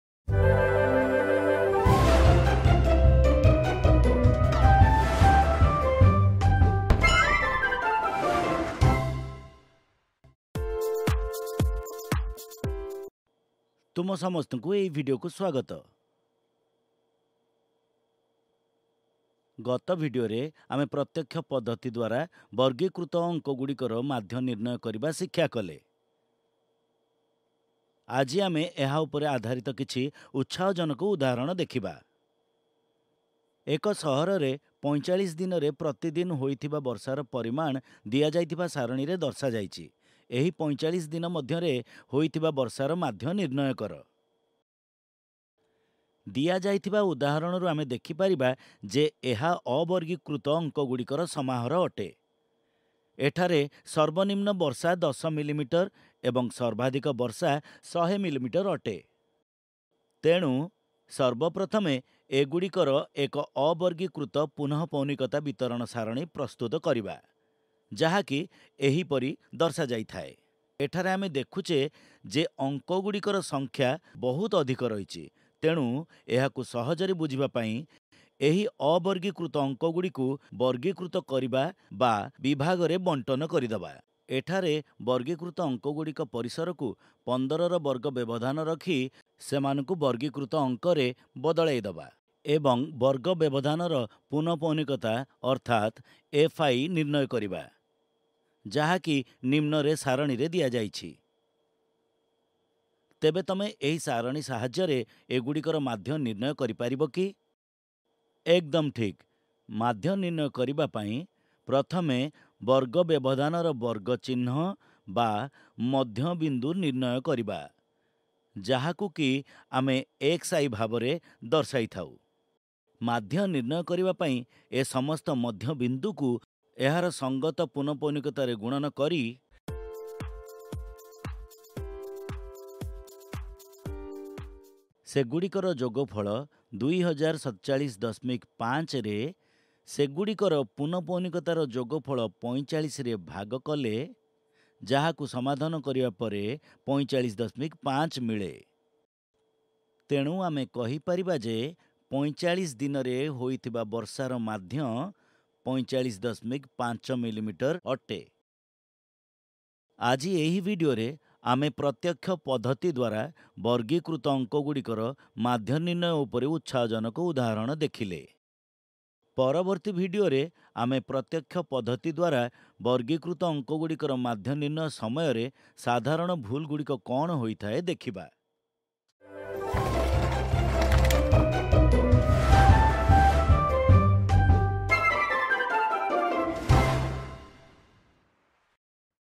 ବର୍ଗୀକୃତ ତଥ୍ୟର ମାଧ୍ୟ- ପ୍ରତ୍ୟକ୍ଷ ବିଧି|Part 2/3|Mean of Grouped Data- Direct Method|Odia|Class 10
Class 10 | ବର୍ଗୀକୃତ ତଥ୍ୟର ମାଧ୍ୟ- ପ୍ରତ୍ୟକ୍ଷ ବିଧି|Part 2/3|Mean of Grouped Data- Direct Method|Odia|Class 10

TicTacLearn Video
Interactive Videos